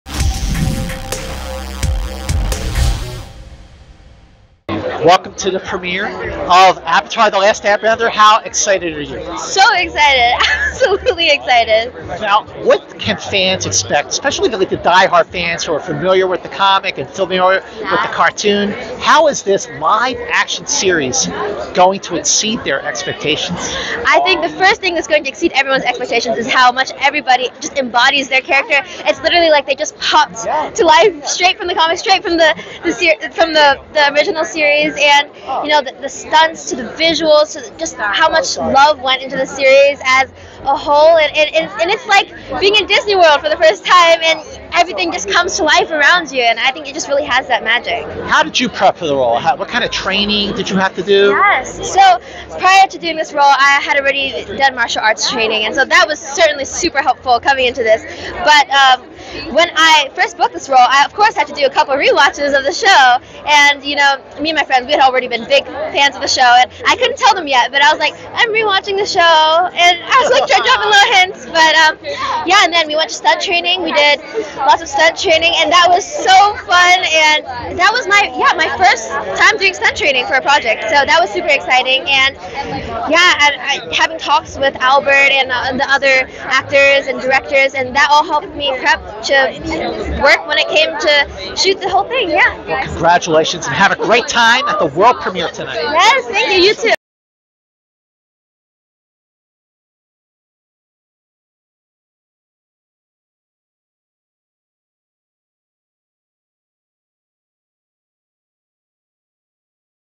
Thalia Tran (Mai) Likens The ATLA Set To Being At Disney World
At the Avatar: The Last Airbender premiere Screen Rant interviews Arden Cho, Daniel Dae Kim & more about the live-action remake of the animated show.

Avatar: The Last Airbender premieres February 22 on Netflix.